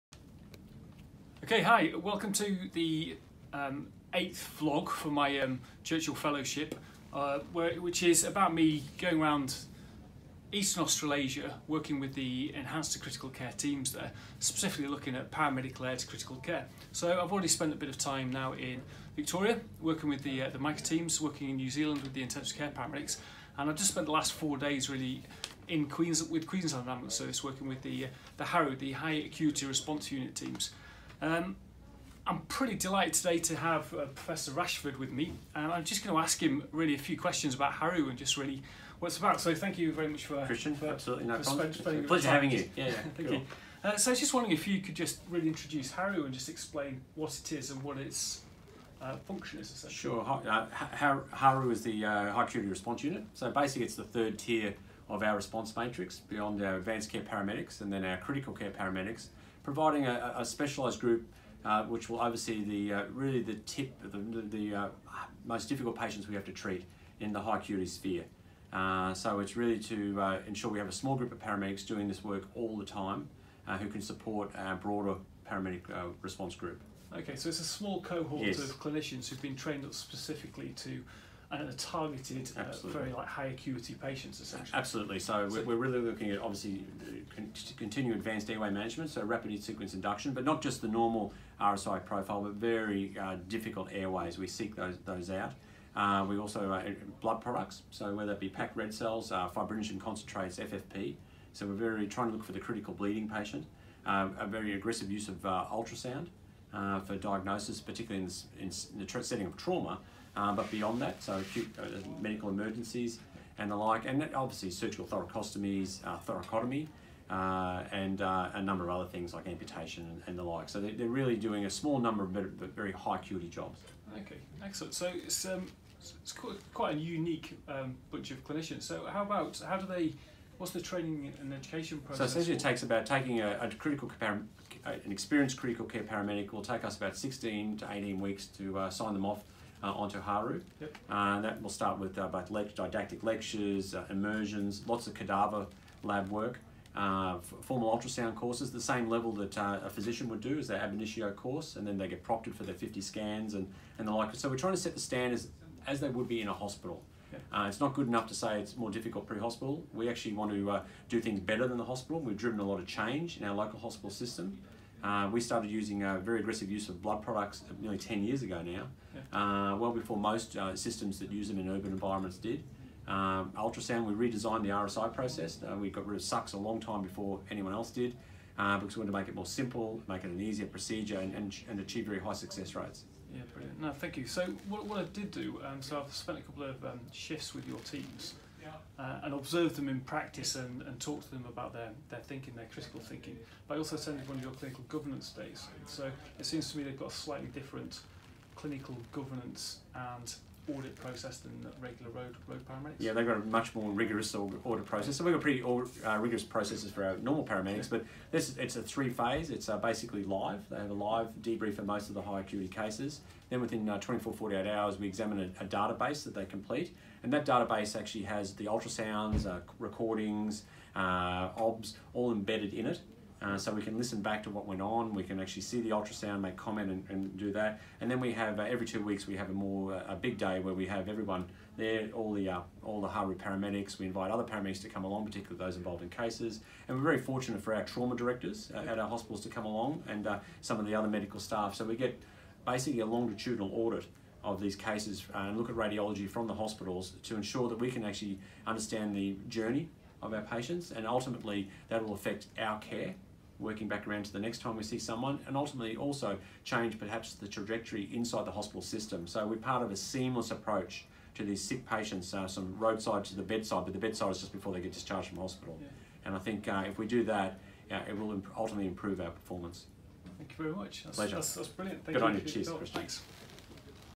VLog#8 chatting to Prof Rashford about the Queensland Ambulance Service HARU
As part of my Churchill Fellowship exploring paramedic led critical care teams in Australasia I had the privilege of working with and learning from the High Acuity Response Units that operate out of Brisbane. This road based, paramedic level service offers patients suffering from life threatening injury or illness the highest level of care. Prof Rashford was the units brainchild and continued to lead the team.